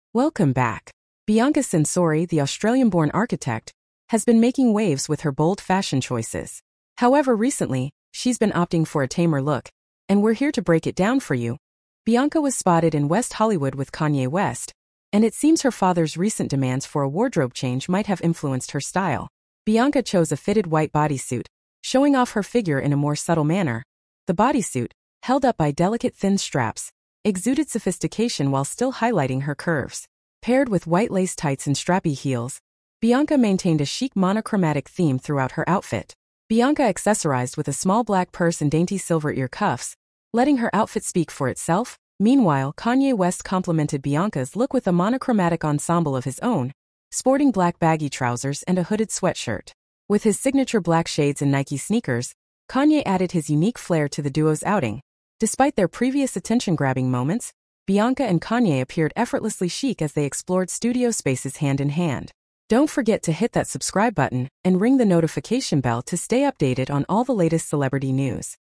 Bianca Censori COVERS UP in a figure-hugging white bodysuit for outing with Kanye West in LA
Bianca Censori ditched her recent jaw-dropping ensembles for a tamer look as she stepped out with Kanye West in West Hollywood, CA.